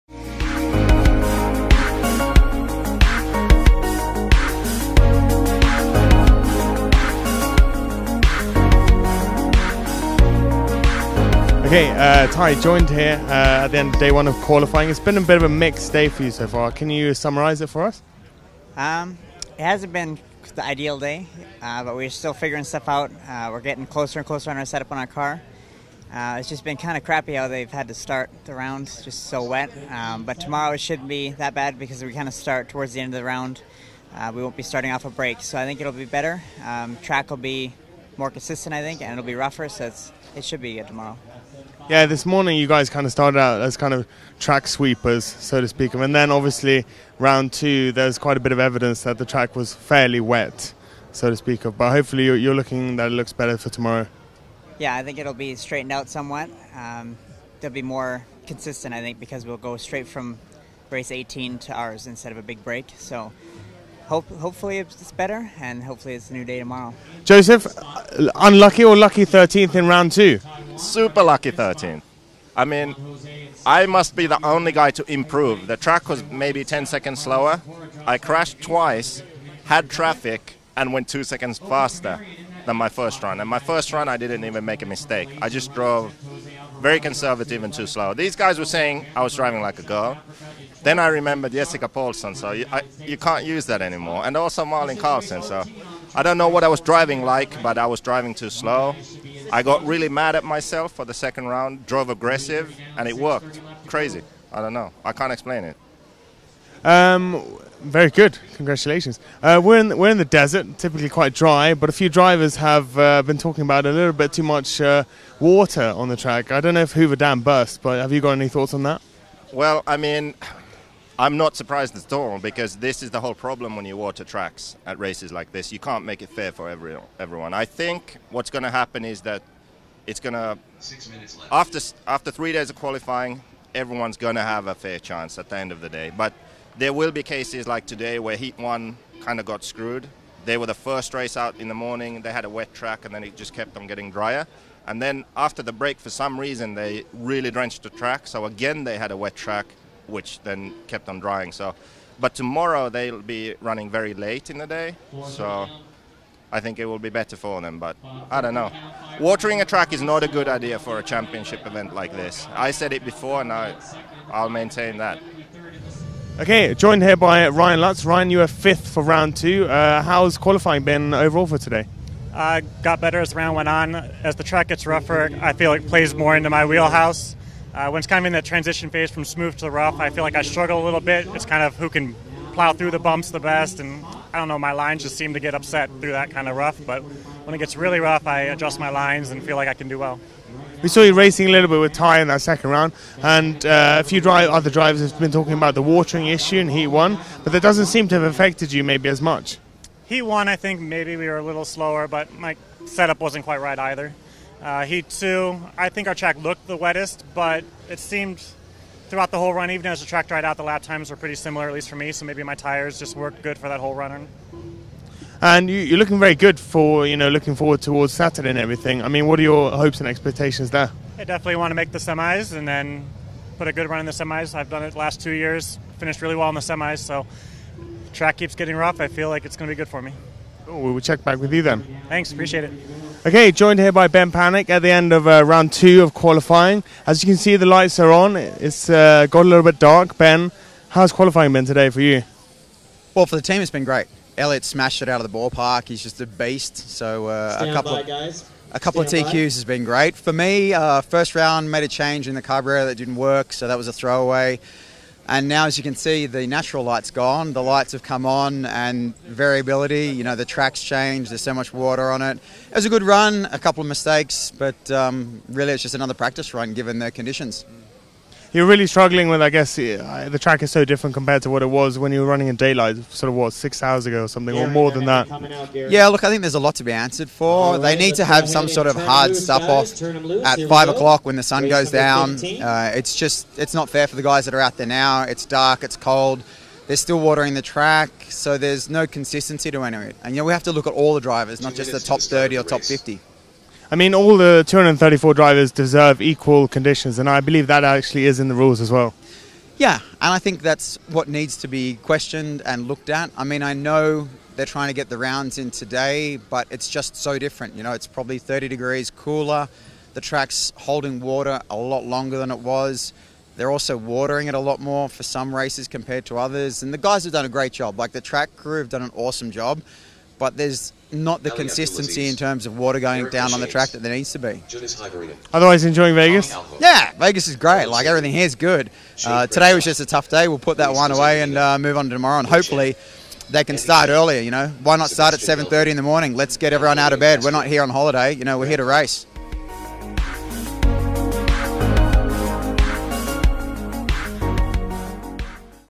2016 IFMAR Worlds -Q2 Driver Catch up
We catch up with Ty Tessmann to get his thoughts on day 1 of qualifying as well as Joseph Quagraine, Ryan Lutz and Ben Panic